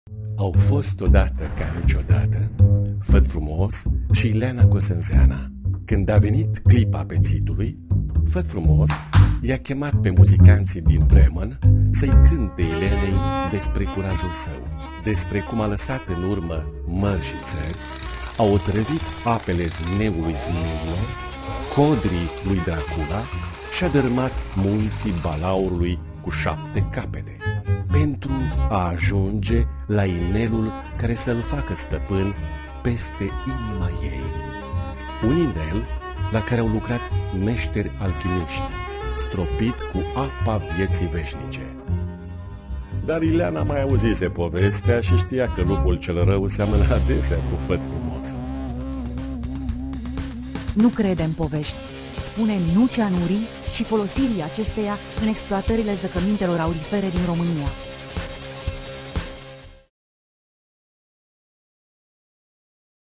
Romania Fara Cianuri - Spune NU Cianurii
Implica-te si voteaza pe impotriva folosirii
cianurii in minerit in Romania.
Spot Video 1min.
Made by Tudor Petremarin